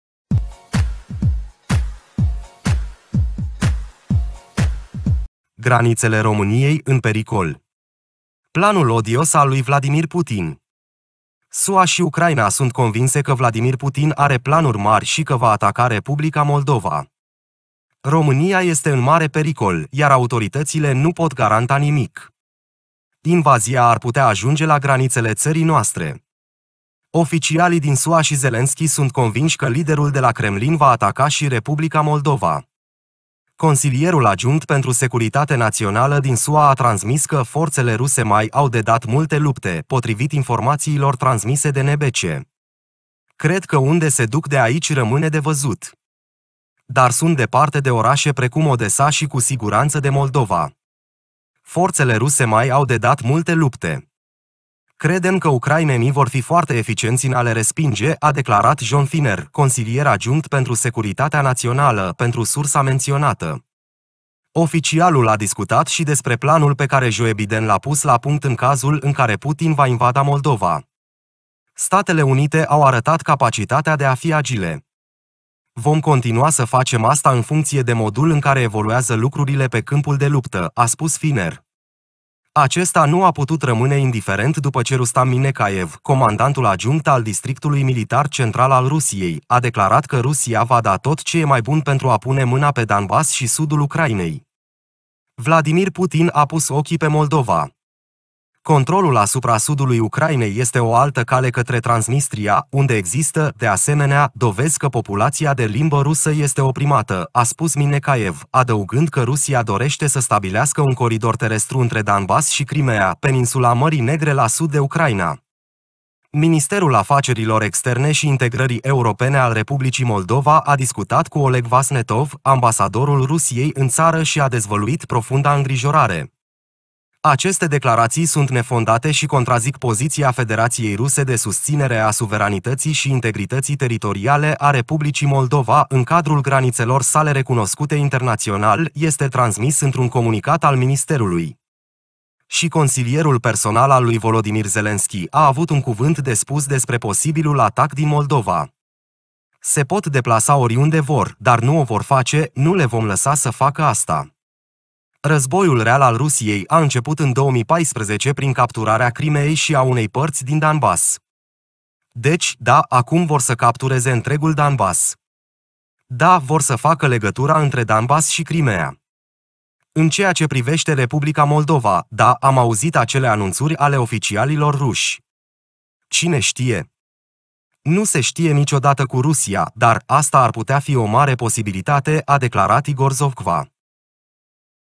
Graniţele României, în pericol?! Planul odios al lui Vladimir Putin
SUA și Ucraina sunt convinse că Vladimir Putin are planuri mari și că va ataca Republica Moldova. România este în mare pericol, iar autoritățile nu pot garanta nimic. Invazia ar putea ajunge la granițele țării noastre.